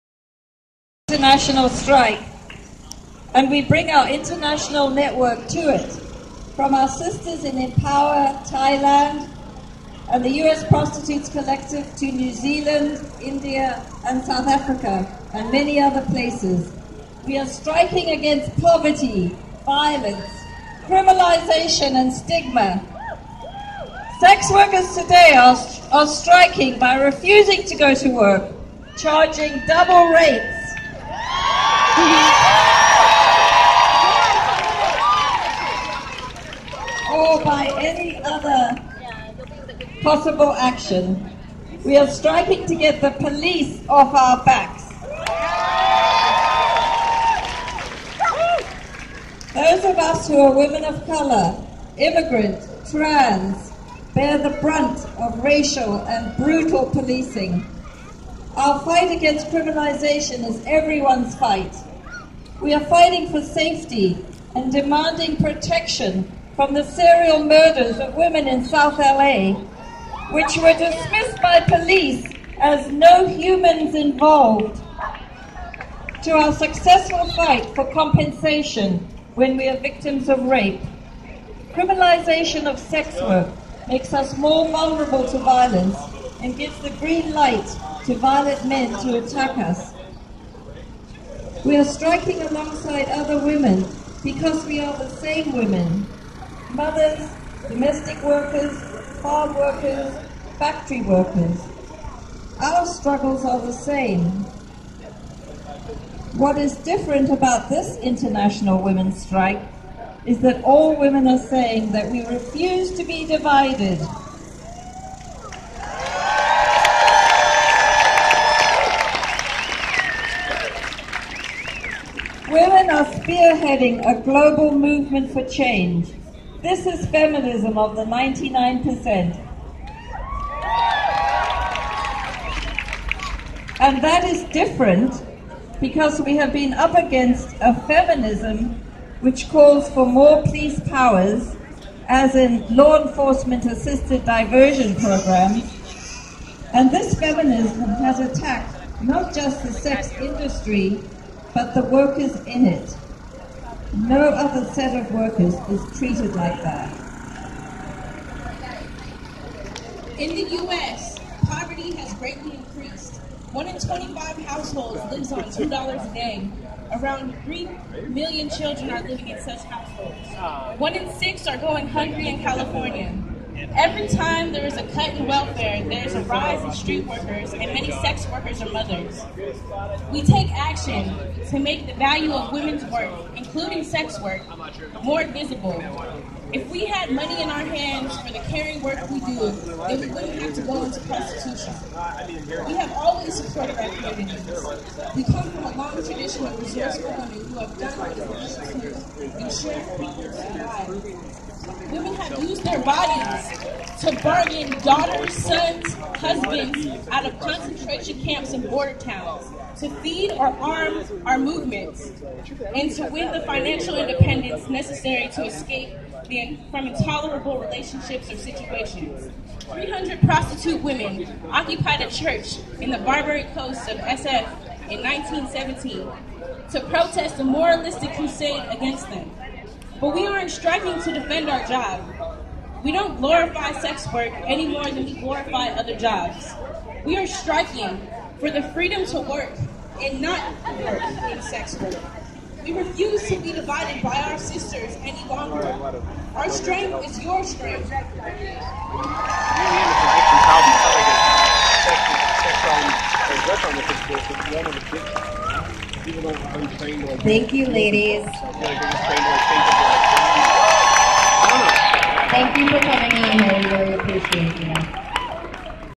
( VID181917 ) Oakland Women's Strike ~Pt.2
( MAR8 / 2017 ) Oakland California
~~ " WHAT WE'RE FIGHTING FOR:
In 46 countries across the world women are striking on March 8th for a better world. In solidarity with the international women's strike, we are issuing a call for women in Oakland to organize ourselves against deportations, workplace harassment, out of control rent, broken healthcare, unlivable wages, discrimination, and police violence.
March 8th will be the beginning of a new international feminist movement that organizes resistance not just against Trump and his misogynist policies, but also against the conditions that produced Trump, namely the decades long economic inequality, racial and sexual violence, and imperial wars abroad."